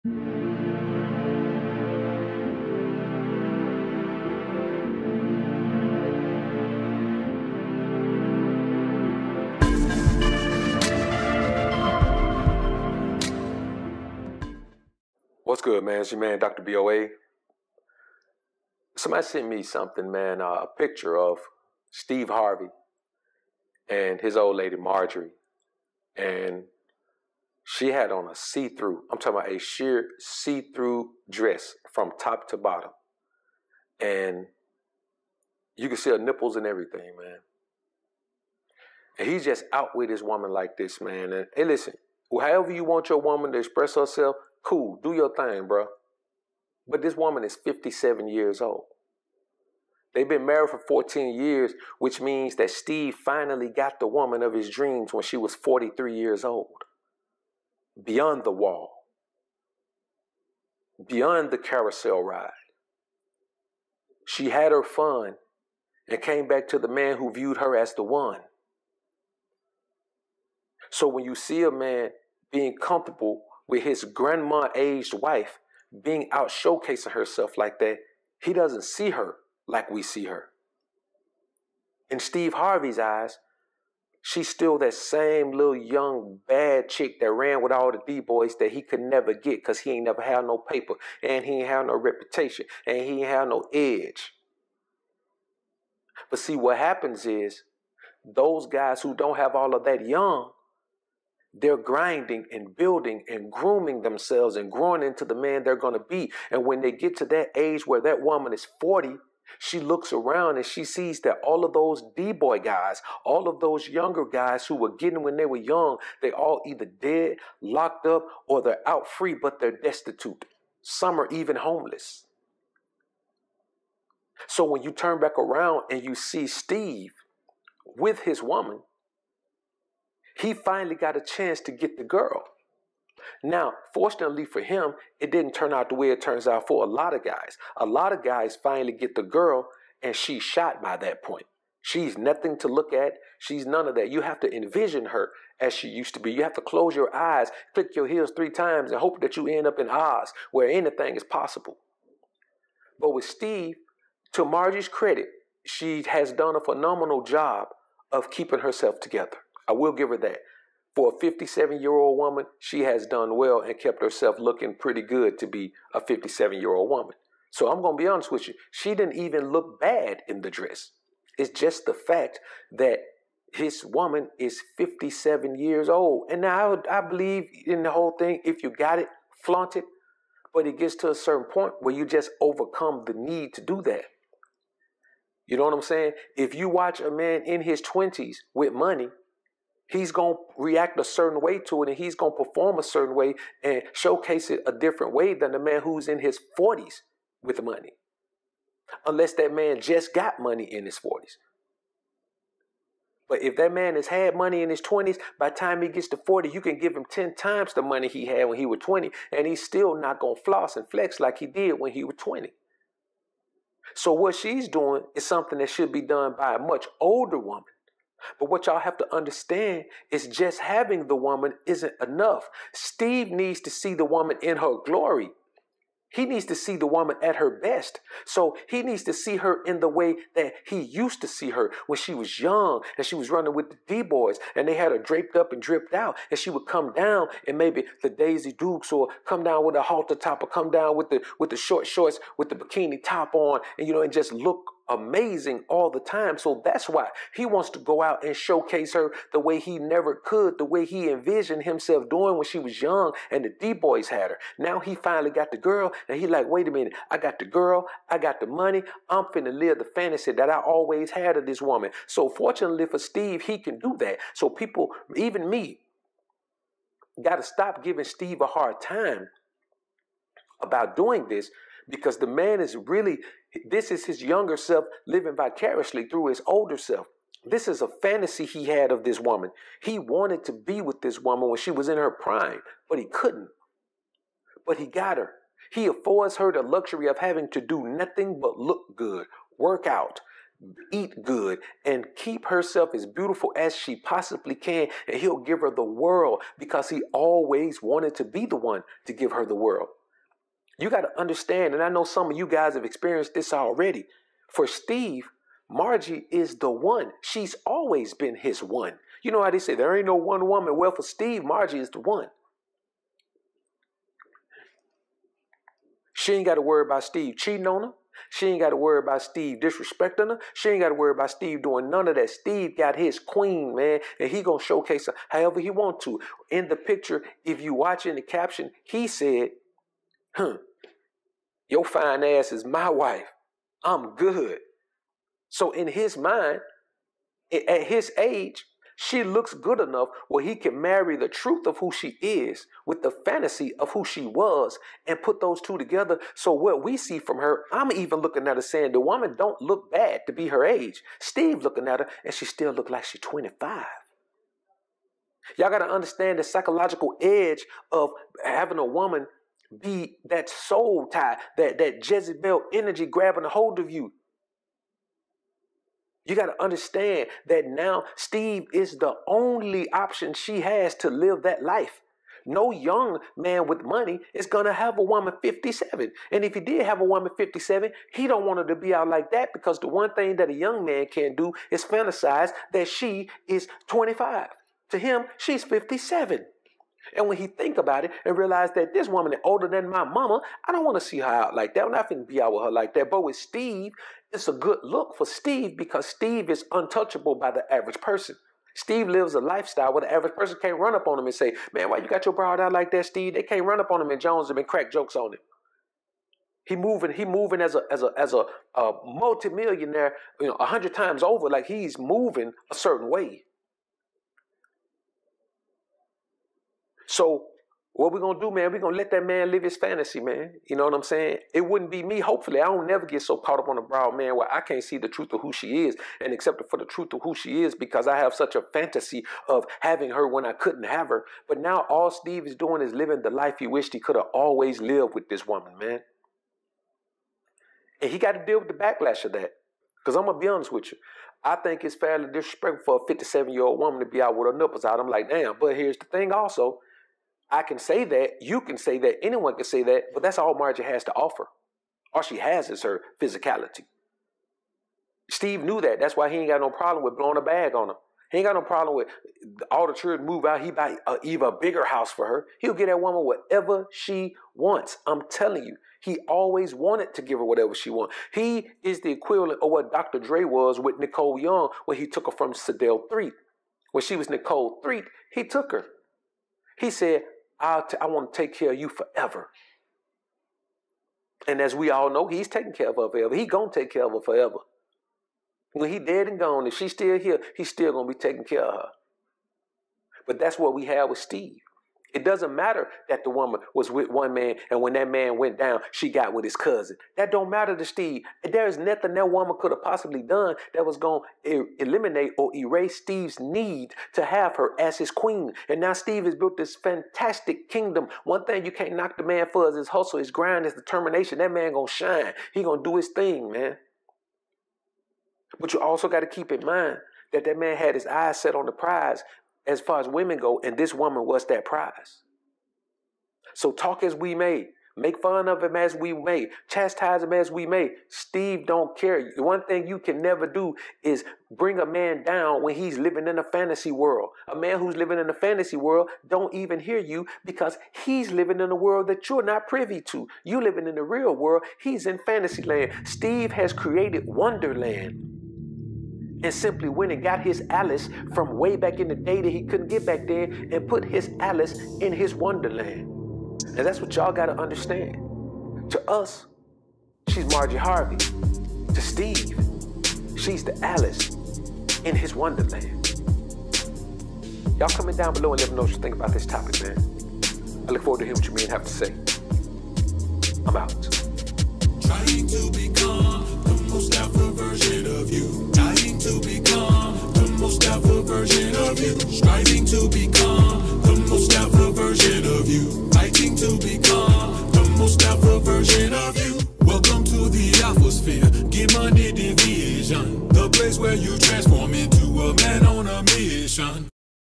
Why STEVE HARVEY Let's His 57yo Grandma Aged Wife MARJORIE Show Her Secrets In a "See Through" Dress
Get the ALPHA MUSIC mixtape
For the best info available on becoming your MOST ALPHA Version